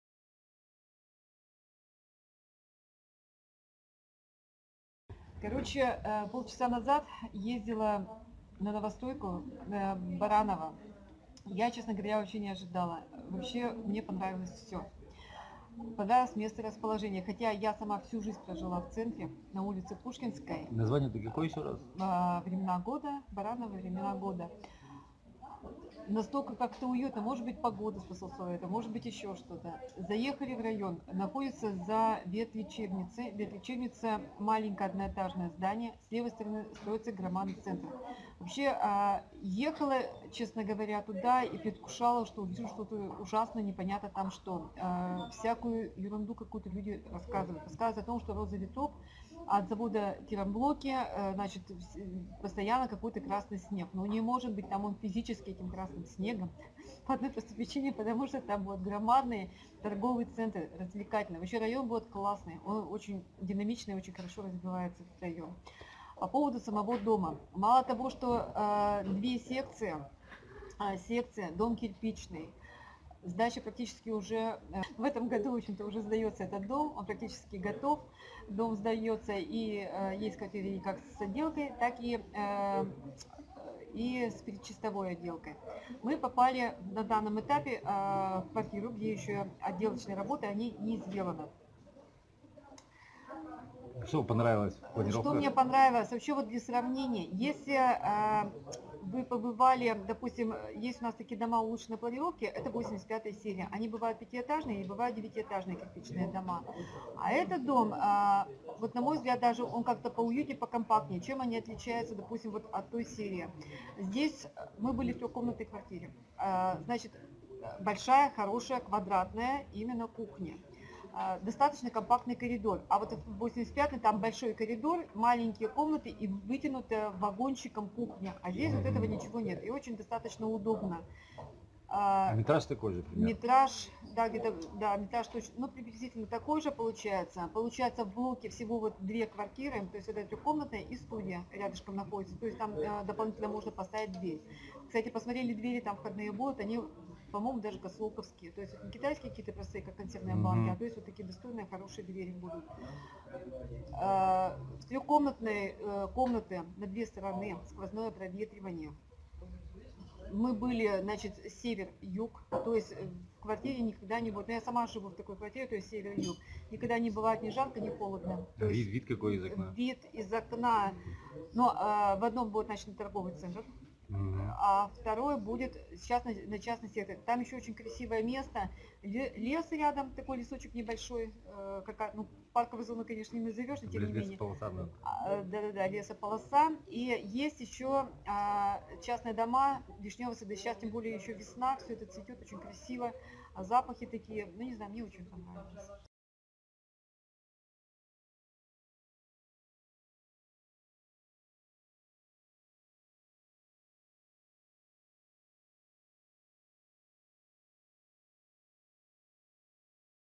ЖК Времена Года Ижевск Отзыв специалиста
ЖК "Времена Года" - Ижевск
Мнение специалиста Центра Недвижимости Ижевска - Надежды Суворовой